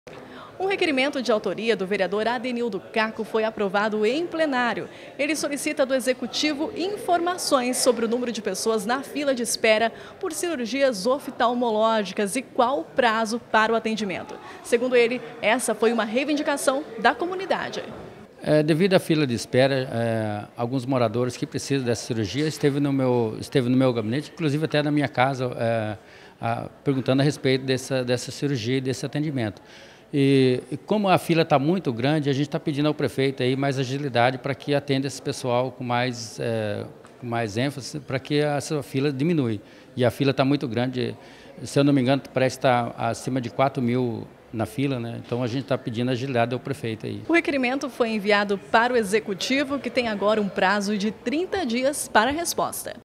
Vereador Kako requer informações sobre a fila de espera para cirurgias oftalmológicas
Devido às reivindicações da comunidade, o parlamentar fez um requerimento solicitando ao Executivo informações sobre a fila de espera para as cirurgias oftalmológicas. O requerimento foi aprovado e aguarda resposta da Prefeitura.